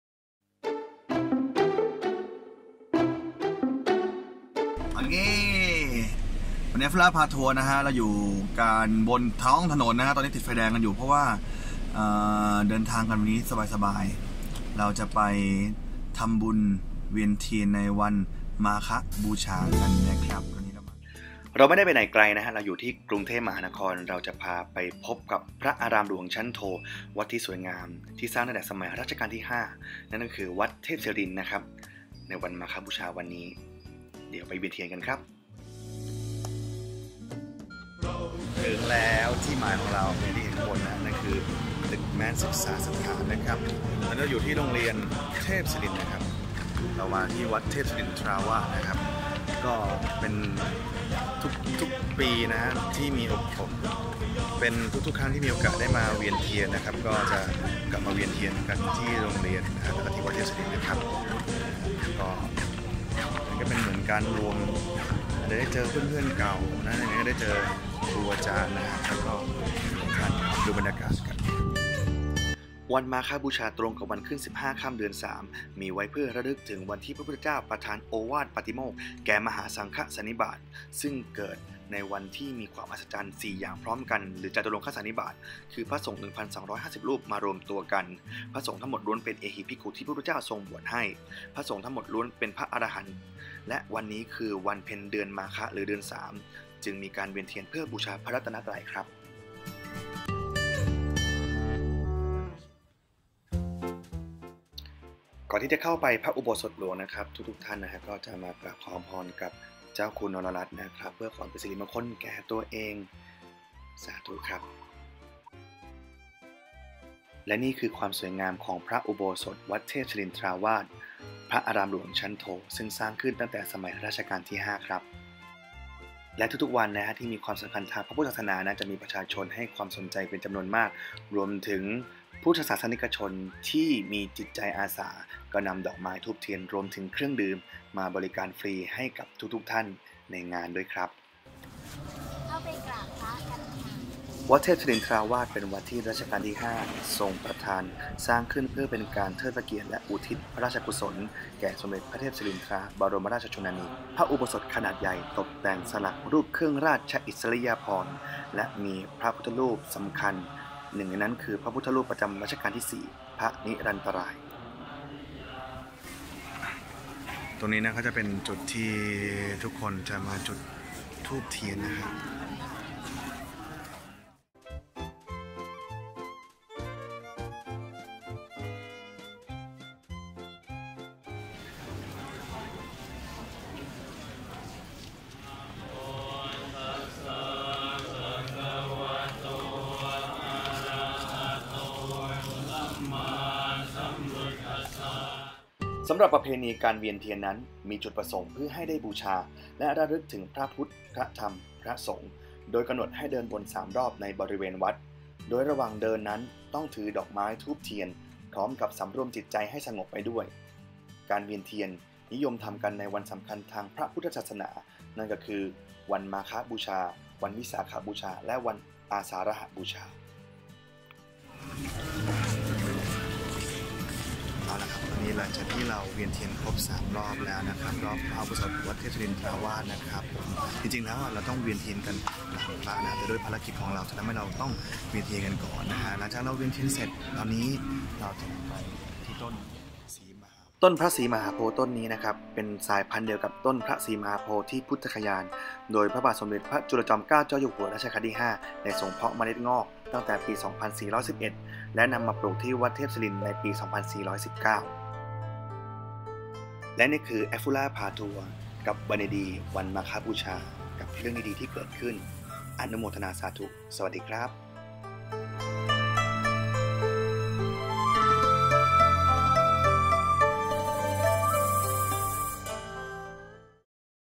apfurapatour ตอน 2 เวียนเทียน วันมาฆบูชา
apfura pa tour ตอนนี้ วันดีๆ กับวันมาฆบูชา มาทำบุญเวียนเทียน ที่วัดเทพศิรินทราวาส ตามประสาเด็กเทพ นักเรียนเก่าที่ต้องย้อนกลับมาที่โรงเรียน เป็นประจำอยู่แล้ววันนี้เลย พาทุกคนมาชมวัดสวยงาม

วัดที่เป็นพระอารามหลวง ชั้นโท วัดเก่าแก่ สร้างตั้งแต่สมัยรัชกาลที่ 5

เป็นวัดที่ ร.5 พระราชทานสร้าง เพื่อเป็นพระราชกุศล ให้แด่ พระมารดา นั่นก็คือ สมเด็จพระเทพศิรินทราบรมราชชนนี ใน รัชกาลที่ 4

และในพระอุโบสถ ยังมี พระนิรันตราย พระพุทธรูปประจำรัชกาลที่ 4ประดิษฐานอยู่ด้วย

ความพิเศษของอุโบสถนี้ คือมีขนาดใหญ่ มีการตกแต่งสวยงาม ด้วยศิลปไทย แบบหยดน้ำ ตามขอบประตู ซุ้มโบสถ์ และยังมีการแกะสลัก เพดานเป็นลายเครื่องราชอิสริยาภรณ์

วันนี้ที่เรามาเป็นมาฆบูชา เป็นวันที่มีความสำคัญทางพระพุทธศาสนา โดยความอัศจรรย์เกิดขึ้นพร้อมกัน 4 อย่าง หรือเรียกว่า จาตุรงคสันนิบาต

คือ

1.พระสงฆ์ 1,250 รูปที่พระพุทธองค์ได้ส่งไปเผยแพร่พระพุทธศาสนาตามแว่นแคว้นต่างๆ ได้กลับมาเฝ้าพระพุทธเจ้าอย่างพร้อมเพรียงกันโดยมิได้นัดหมาย
2.พระสงฆ์ทั้งหมดล้วนเป็นเอหิภิกขุที่พระพุทธเจ้าทรงบวชให้ด้วยพระองค์เองทั้งสิ้น ซึ่งเรียกว่า เอหิภิกขุอุปสัมปทา
3.พระสงฆ์ทั้งหมดล้วนเป็นพระอรหันต์ คือผู้ได้อภิญญา 6 ข้อ
4.วันที่พระสงฆ์ทั้งหมดมาชุมนุมกันนี้ตรงกับวันเพ็ญเดือนมาฆะ (วันขึ้น 15 ค่ำ เดือน 3)

ซึ่งในโอกาสนี้ สมเด็จพระสัมมาสัมพุทธเจ้ายังได้ทรงแสดงพระธรรมเทศนาที่เรียกว่าโอวาทปาฏิโมกข์ อันเป็นหัวใจของพระพุทธศาสนาอีกด้วย